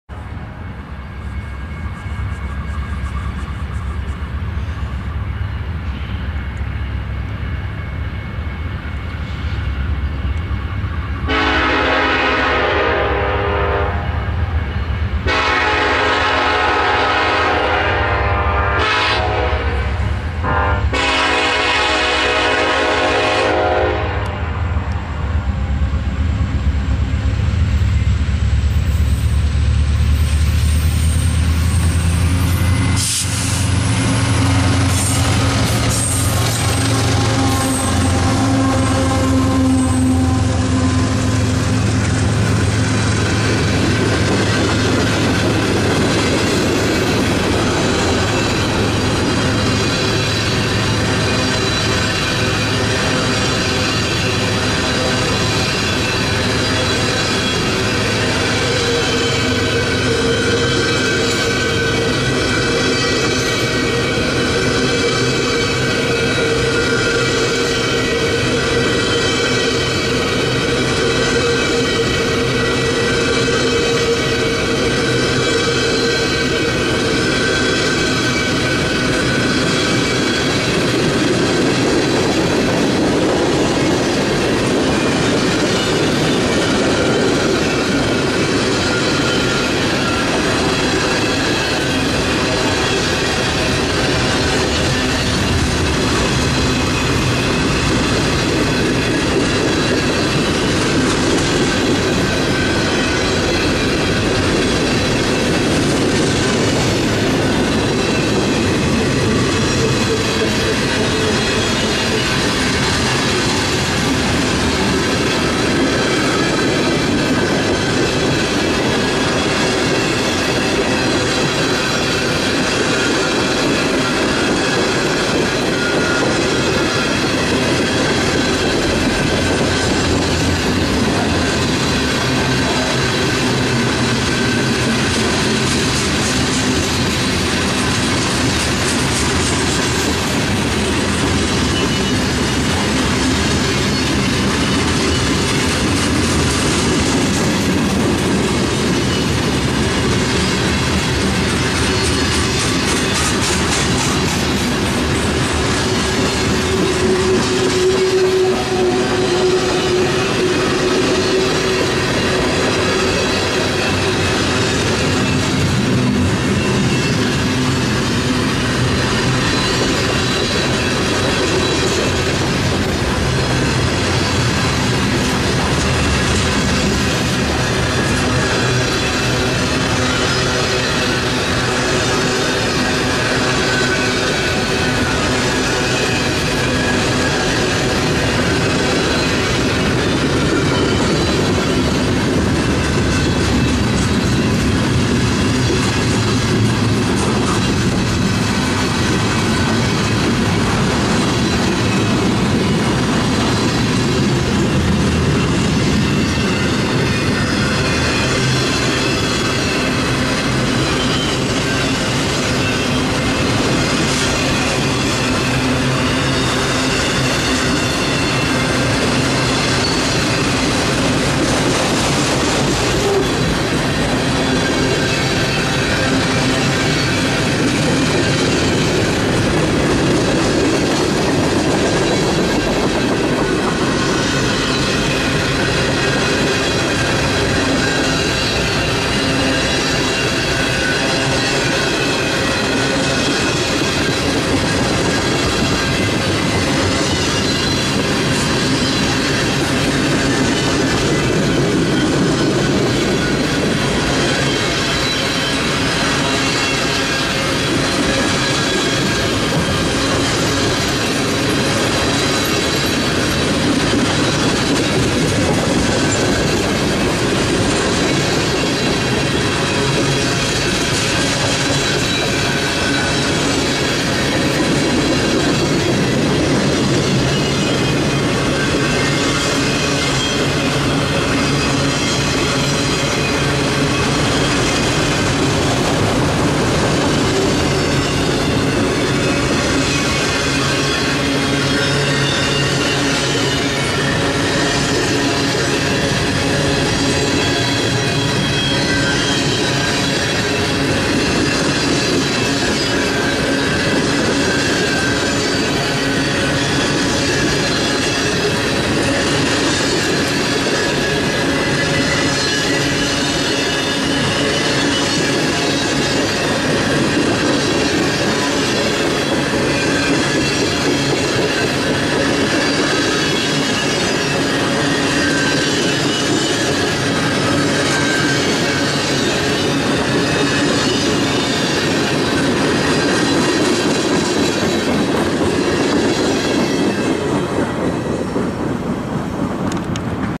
BNSF 8485 (SD70ACe) w/ CP Power, Bettendorf, IA, 11/19/19
A BNSF ACe & CP 8719 lead CP's 475 freight north on the Davenport sub.